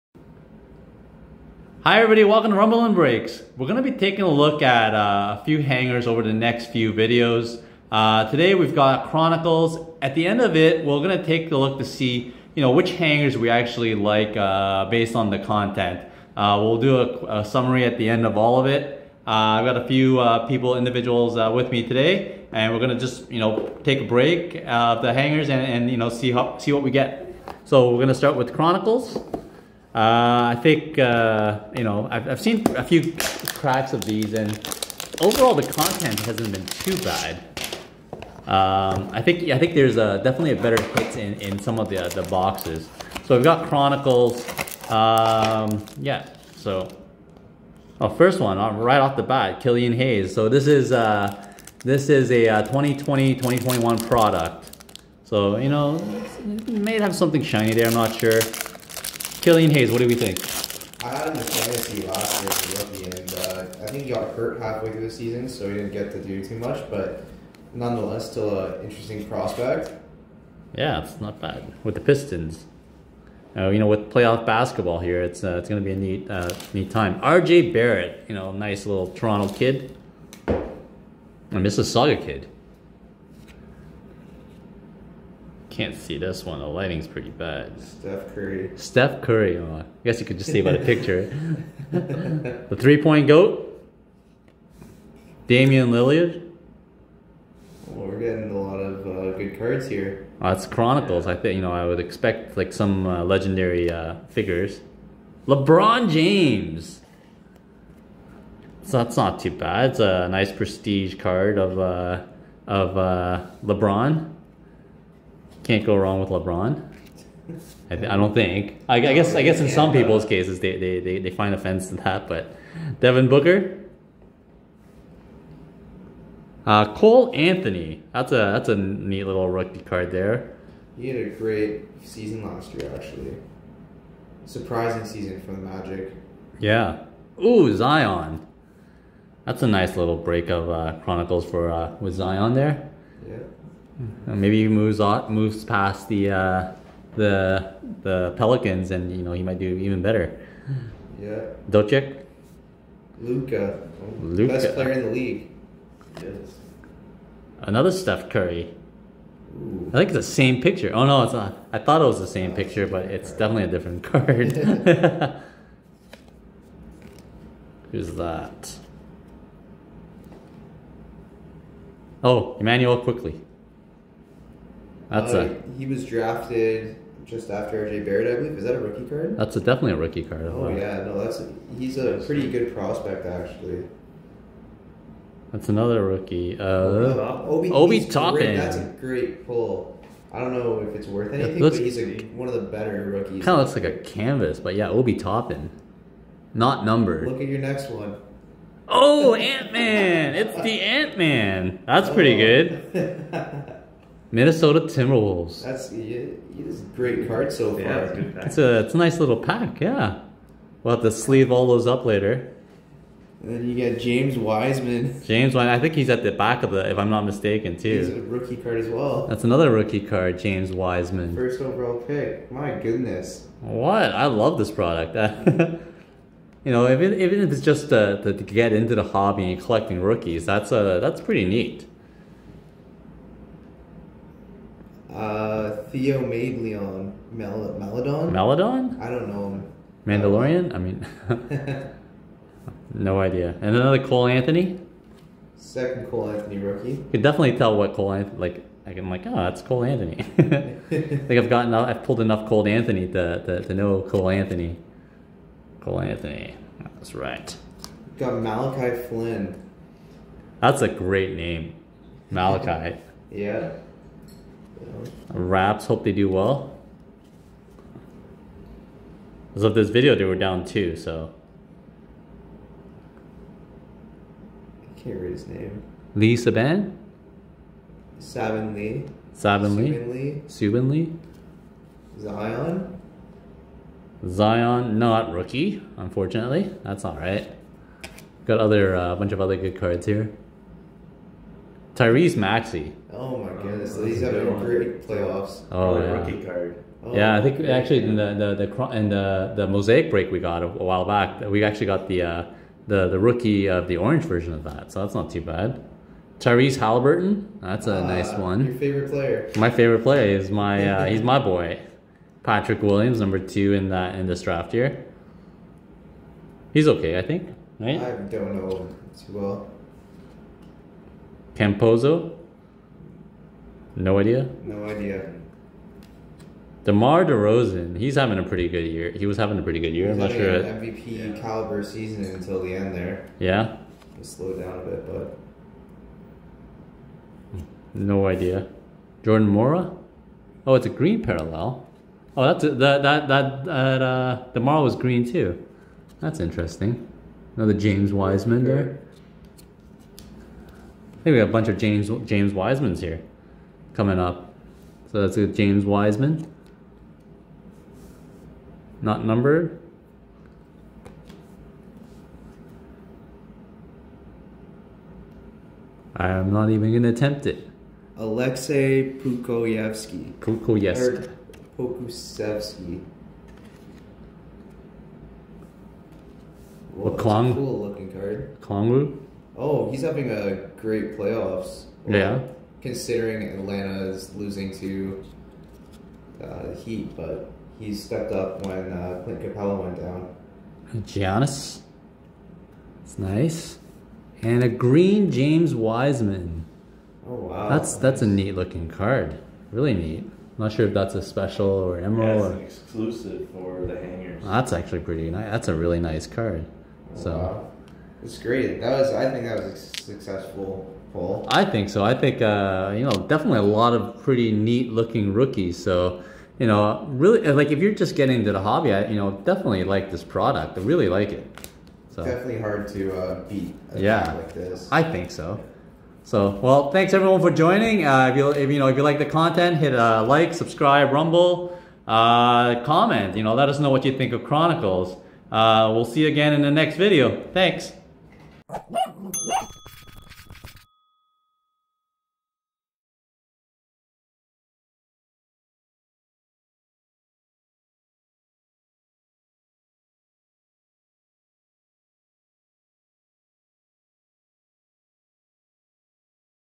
2020-2021 NBA Chronicles Hanger Break: Rookies Galore!
Welcome to Episode 8 of our weekly break. Today we'll be checking out a series of basketball hangers. We're looking to see which could potentially bring value, but also for the pedestrian collector like us who just enjoy collecting cards, what would be the most fun product. We wanted rookies, well we sure got them.brbrFor the average collector of rookies, this is a great product. Personally we really had fun breaking this.brbrWe hope you enjoy it as much as we had enjoyed breaking it!